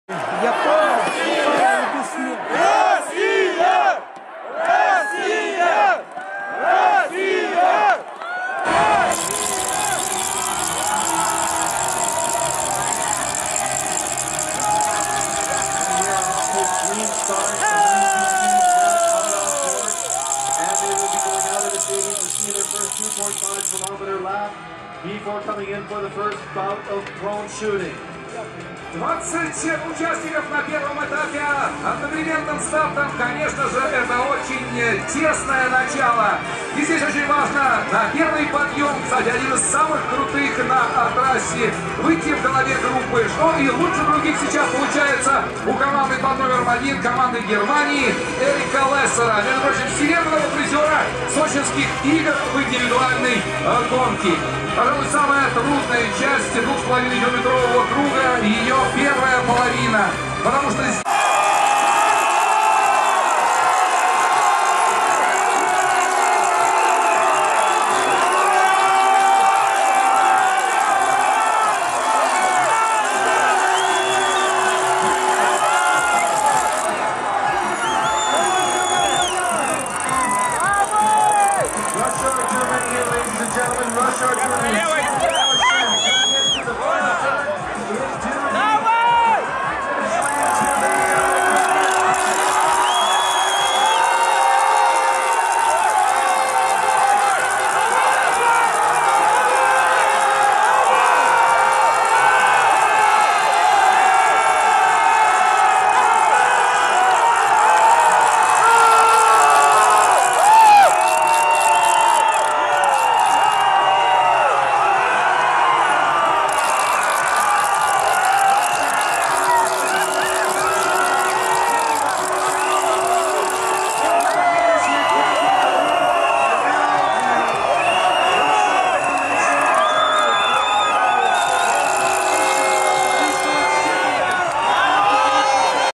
Sochi 2014, biathlon, men's relay
Мужская сборная по биатлону выигрывает эстафет на Олимпийских играх в Сочи 2014.